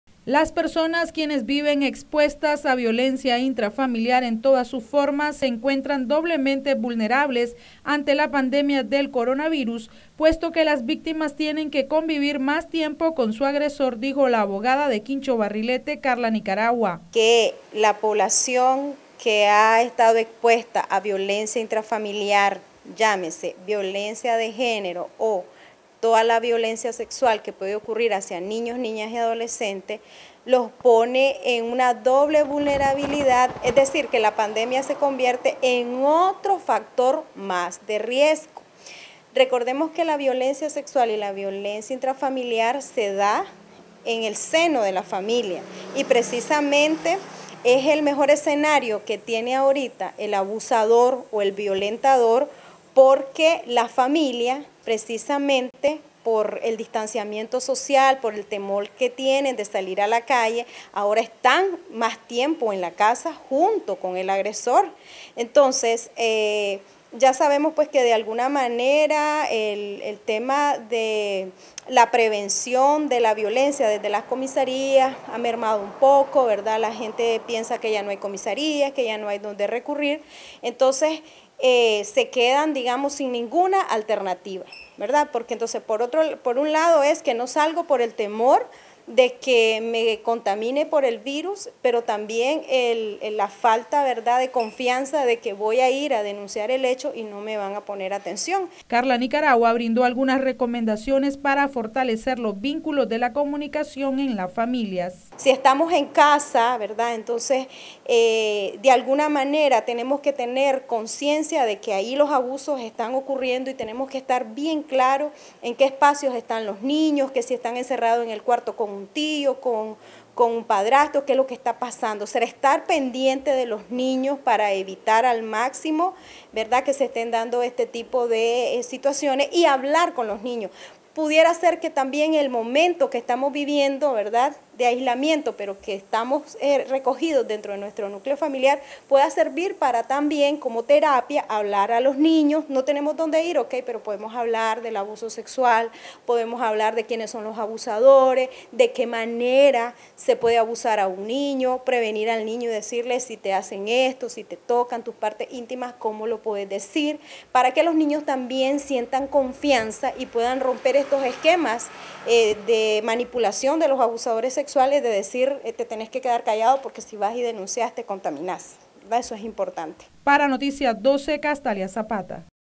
Personas quienes viven violencia son más vulnerables en el aislamiento social
Personas quienes viven violencia son más vulnerables en el aislamiento social, dijo la abogada Karla Nicaragua.